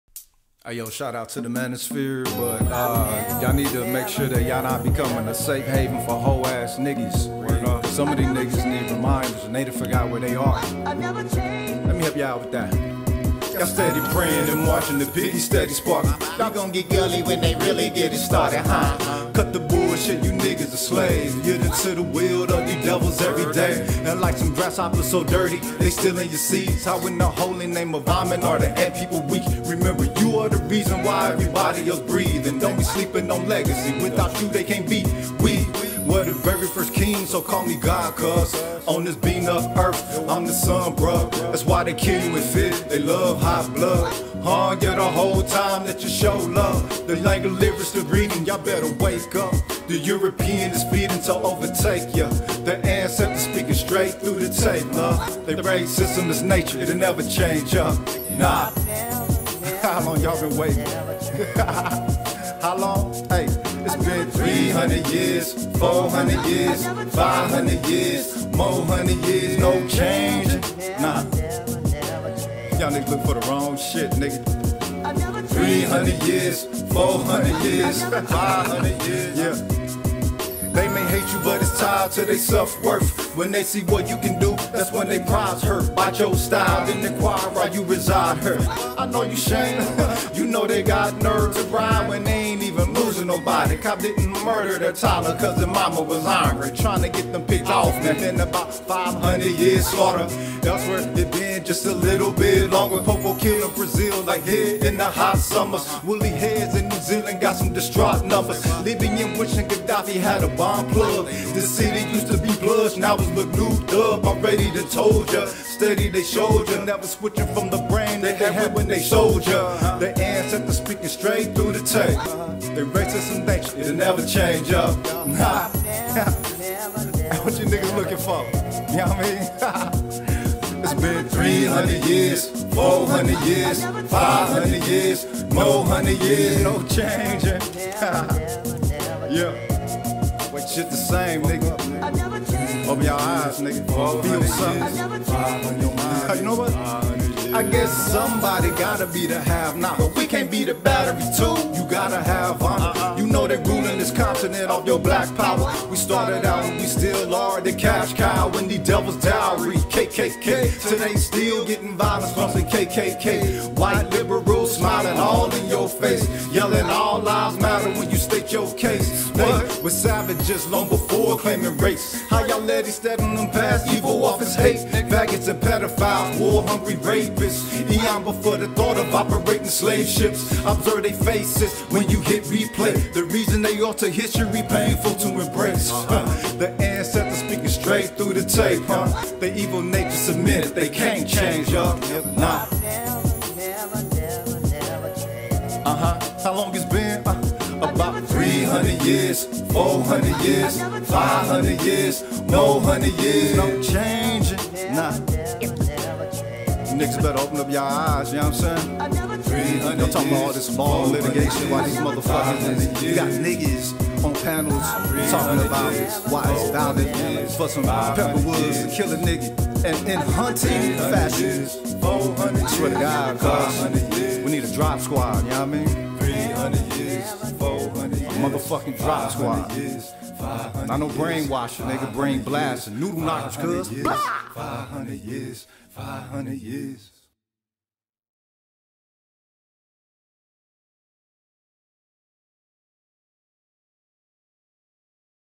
"Emphatically No"
Seems that the Manosphere is an inadvertent safe haven for automatonic coon minded "select" men who run their "elevated" programming better than the "pookie and ray rays" they claim to be stark contrast of.

These niggaz need some reminders that the system that they champion are turning its nose to them the same as they to the less refined cats not in white collar world.